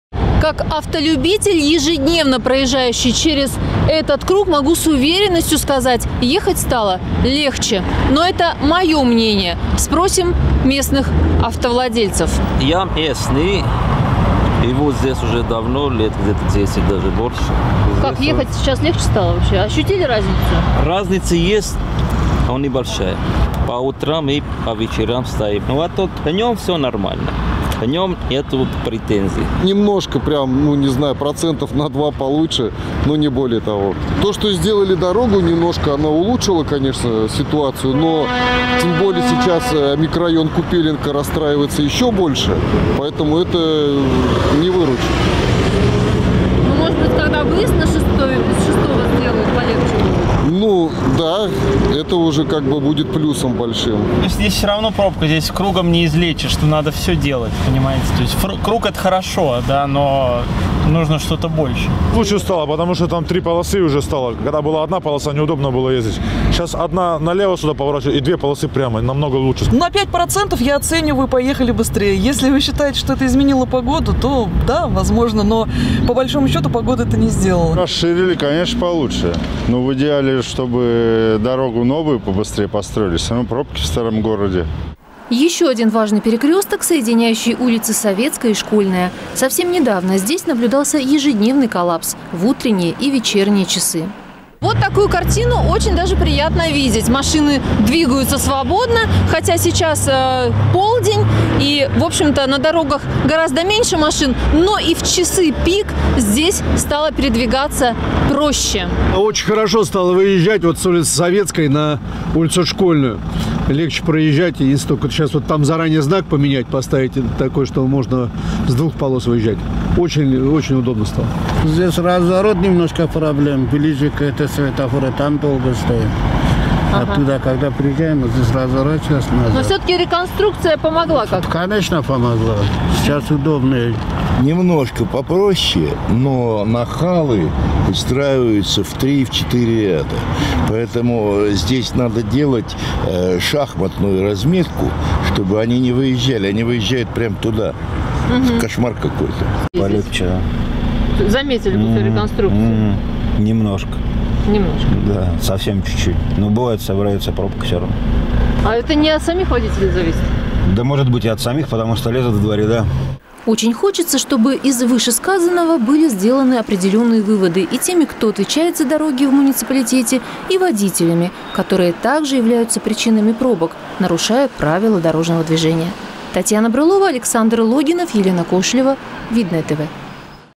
«Видновчане об этом» - мнение жителей о проведенных реконструкциях дорог в городе
Завершены работы по реконструкции развязки на ПЛК в районе пожарной части и выезда с Советской улицы на Школьную. В рубрике «Видновчане об этом» мы решили узнать у местных автовладельцев, насколько улучшилась дорожная ситуация в этих местах.